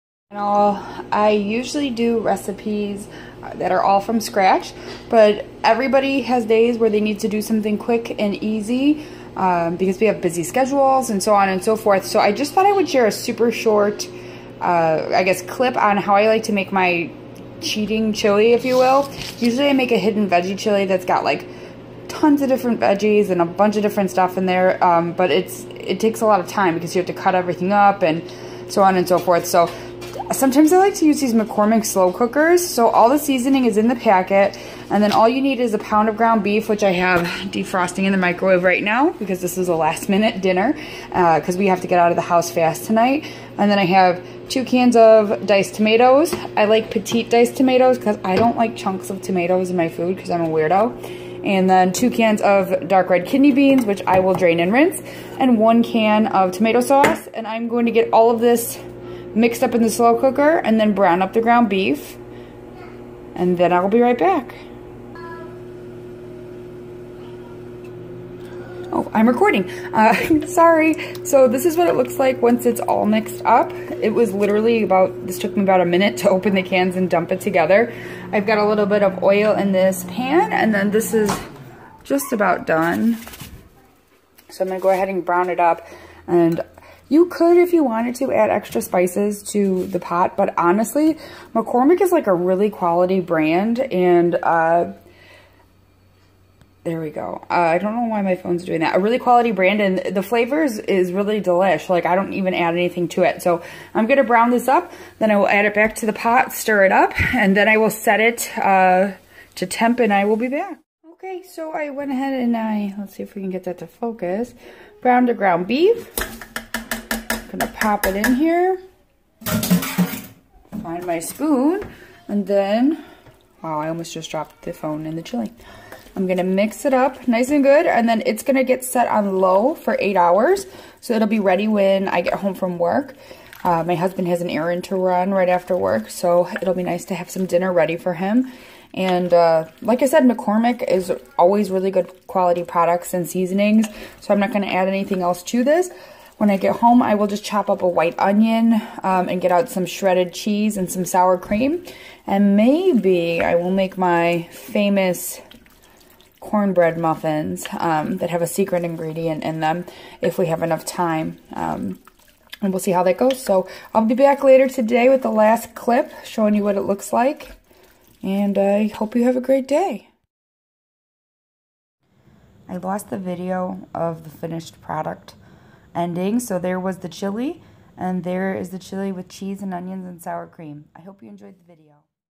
10 Minute Crock Pot Chili - an easy life hack using McCormick Slow Cookers Seasoning
Everyone needs a pantry meal that can be in the crockpot in 10 minutes and on the table when you get home from work. Don't get me wrong... I love homemade chili from scratch. Who doesn't? But every now and then I need a simple, fast, easy clean up type of meal that fits our busy schedule.

This is more of a review and how to... not a recipe.